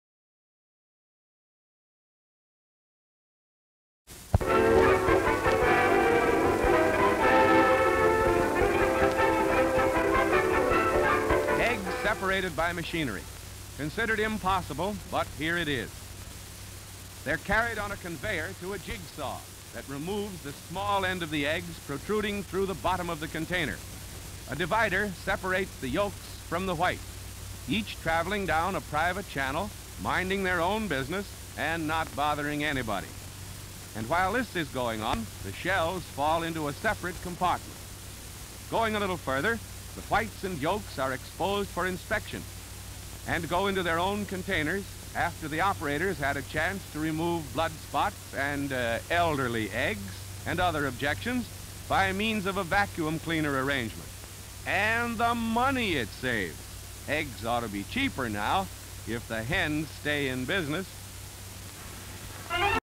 Invention of egg breaker and separator. HD Stock Footage
Invention of egg breaker and separator.

Invention of egg breaker and separator machine. Eggs run through a jig-saw, pulled through a vacuum in the factory. Eggs then pumped into a reservoir. Shells are discarded followed by diversification of yolk and egg-white into different compartments. Location: Modesto California. Date: February 25, 1932.

57,000+ broadcast-quality historic clips for immediate download.